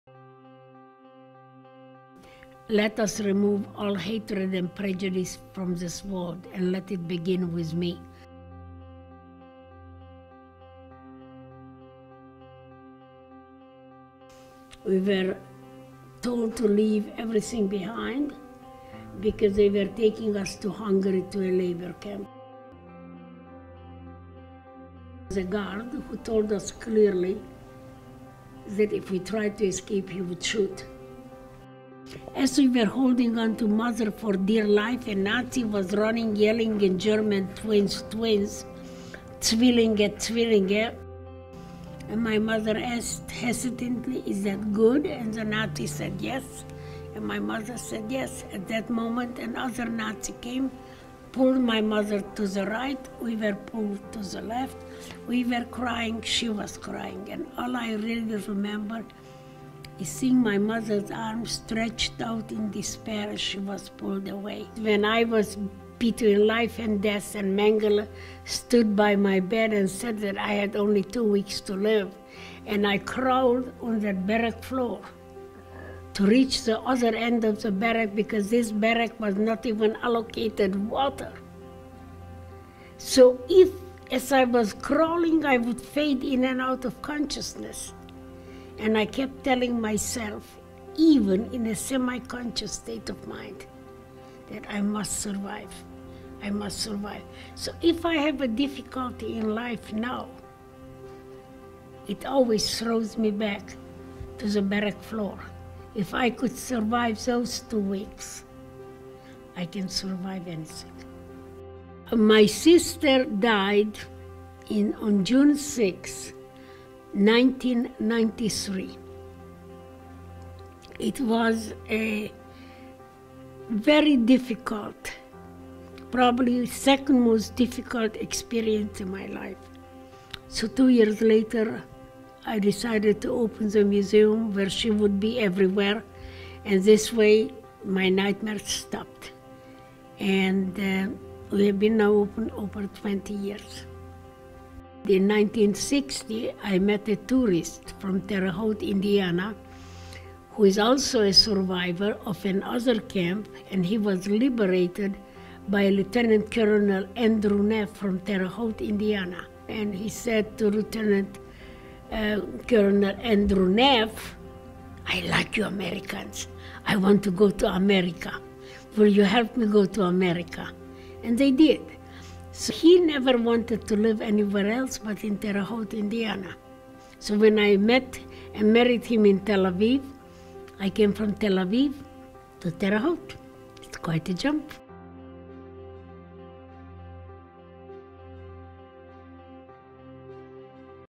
Eva Kor's journey to Terre Haute
A Short documentary of Eva Kor and her struggles and what brought her to the United States, where she opened the CANDLES Holocaust museum.